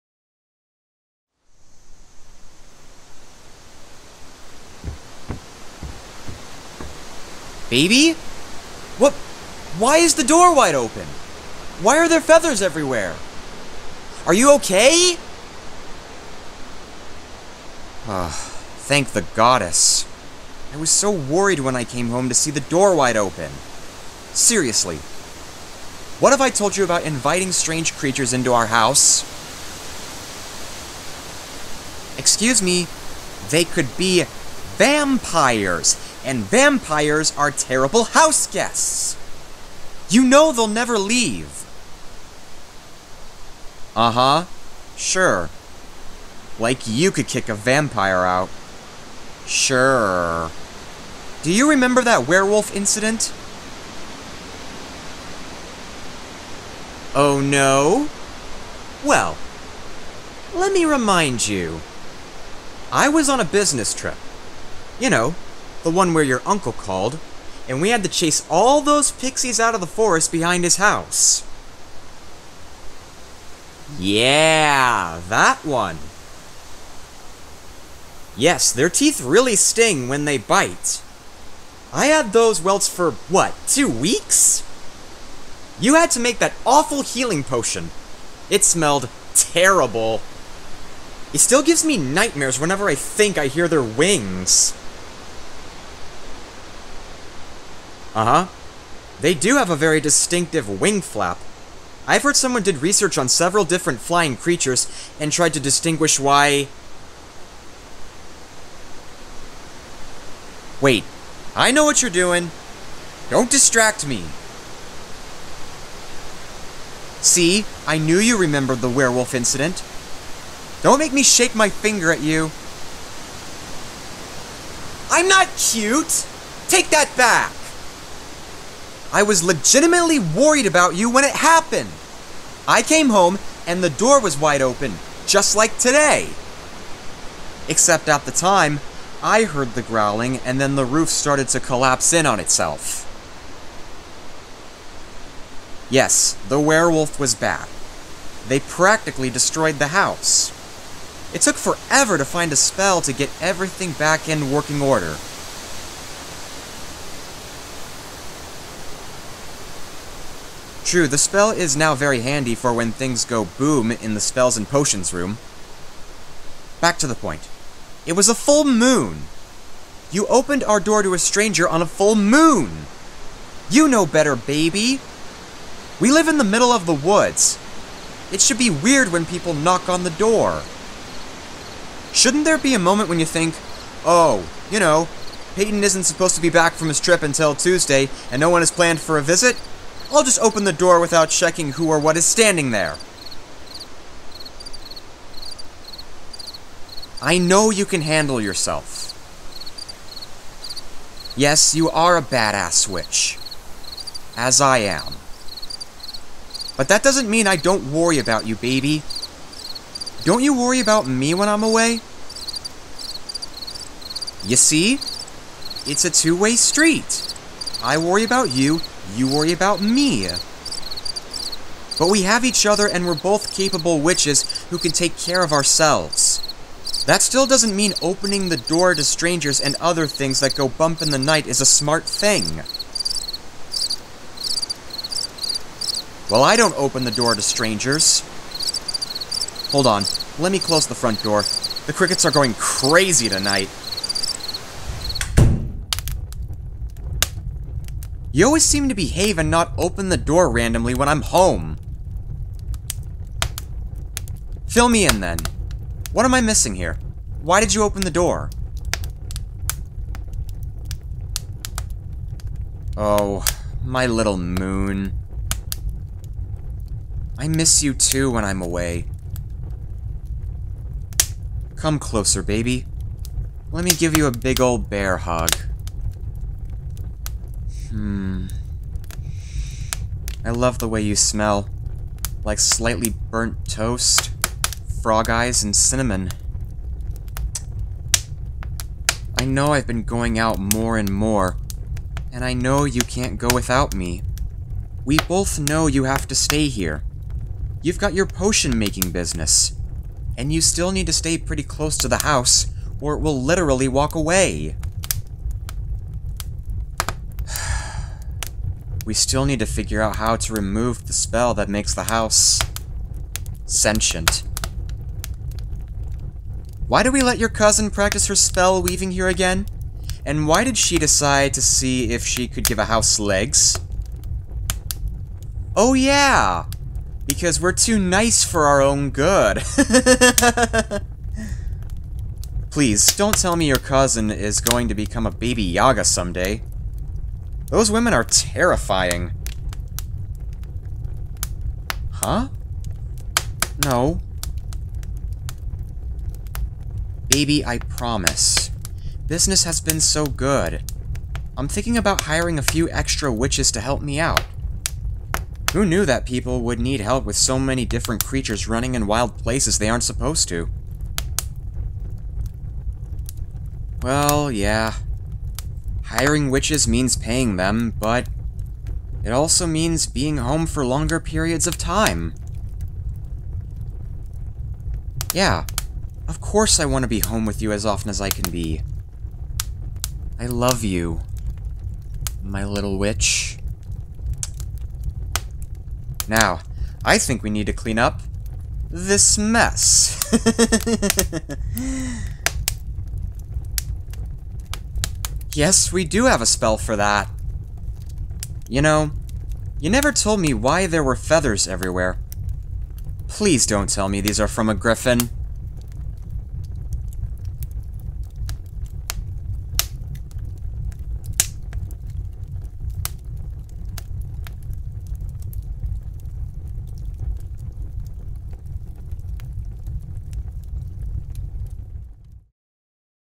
A Conflict With Your Boyfriend Witch | ASMR/Audio RP | (M4F) (Fantasy) (Cute) (Halloween) 🧙🏻‍♂️
A young witch couple live out in the middles of the woods. in a sentient house. The male travels a lot for his job, and always comes home to find his girlfriend into some kind of trouble! This time there was a magical creature that did some kind of rampage. The male witch finds out his girlfriend misses him greatly and they talk about it.

Credits:
Character drawn by me
Script by KrystalWrites on Reddit
SFX used from Storyblocks.com with permission